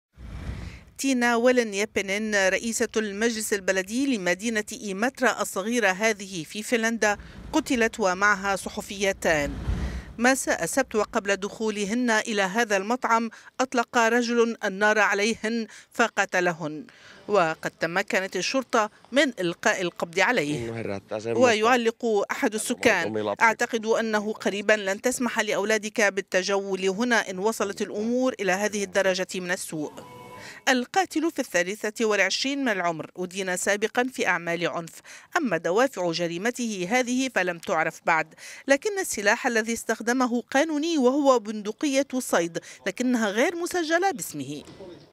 فنلندا: مقتل صحفيتان ورئيس مجلس احدى البلديات
فنلندا التي تعتبر نسبة الجريمة فيها اقل من غيرها من دول العالم، كانت مدينتها ايماترا مسرحاً لجريمة اوقعت ثلاث ضحايا.

مساء السبت، اثناء خروج تينا ويلين - يابينين رئيسة المجلس البلدي وهي اشتراكية ديمقراطية وبرفقتها صحفيتان من احد مطاعم المدينة، اطلق رجل النار عليهن فقتلهن على الفور.

ويستنكر احد السكان قائلاً "اعتقد انه قريباً لن تسمح لاولادك بالتجول هنا، إن وصلت الامور الى هذه الدرجة من السؤ. إنه امر مؤسف".

على الفور القت الشرطة القبض على القاتل. إنه رجل في الثالثة والعشرين من العمر،  أدين ساب…